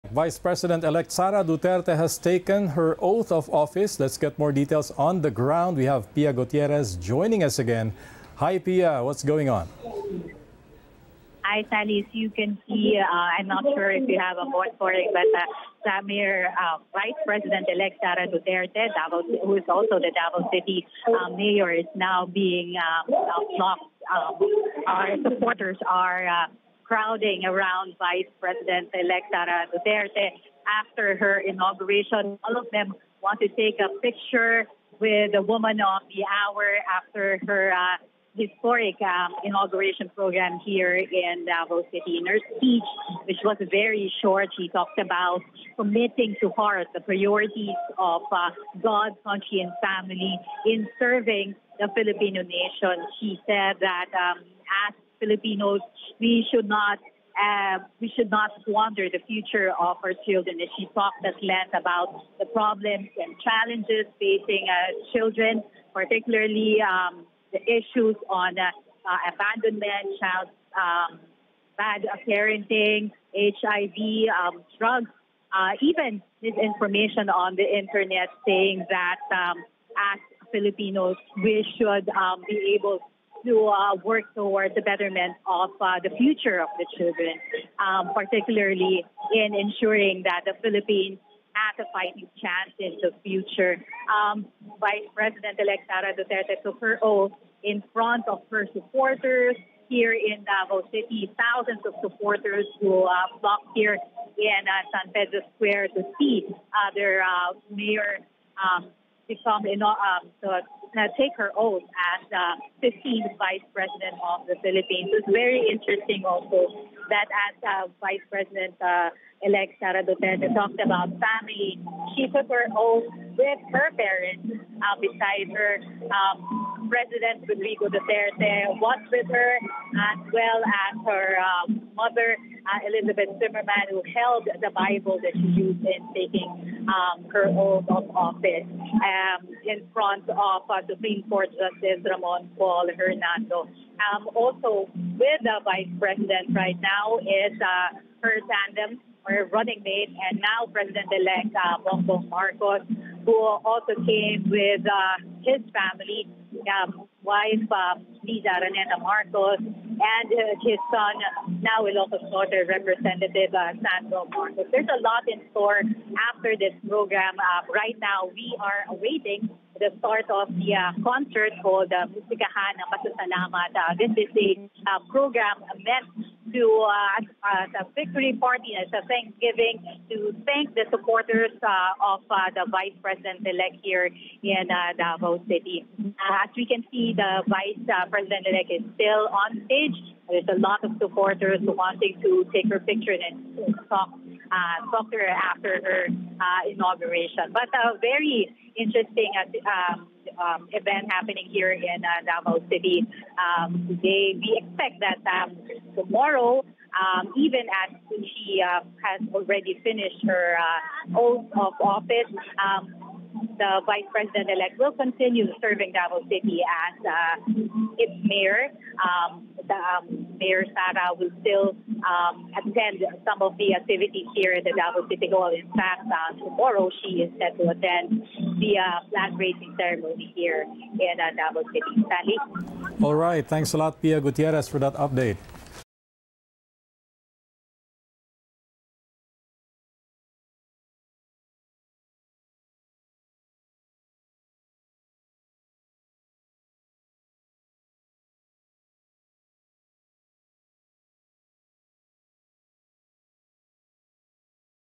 VP-elect Sara Duterte accommodates supporters for photo opportunity | ANC
The Duterte Inaugural: Sara Duterte takes her oath as the 15th vice president of the Philippines at the San Pedro Square in Davao City.